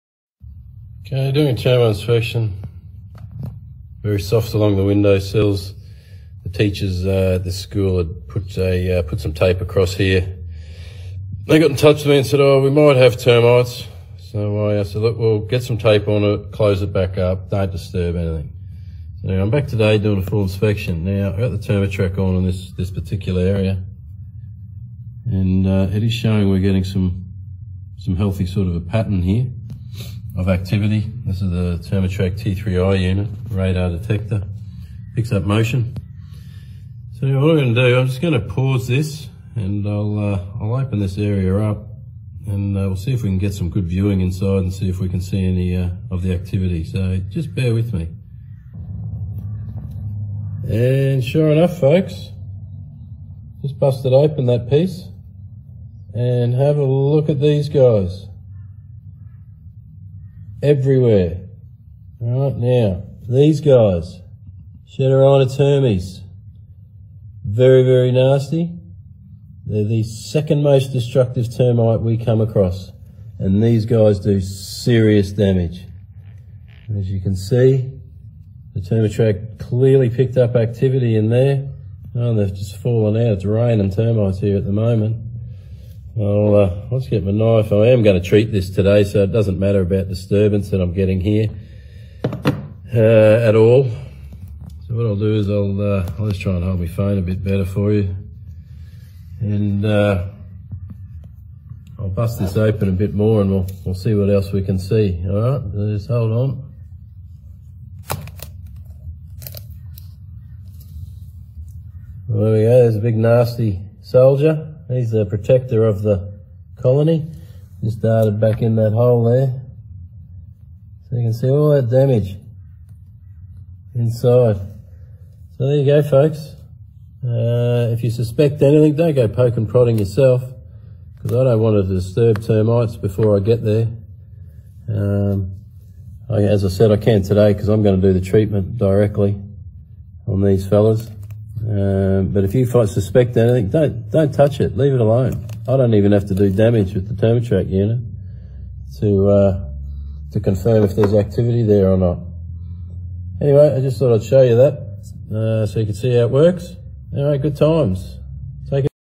Termatrac T3i Radar Sensor non-invasive detection of Termite Activity!
Watch how Noel from Auswise Pest Control detects some serious termite activity with the T3i Radar sensor at a school!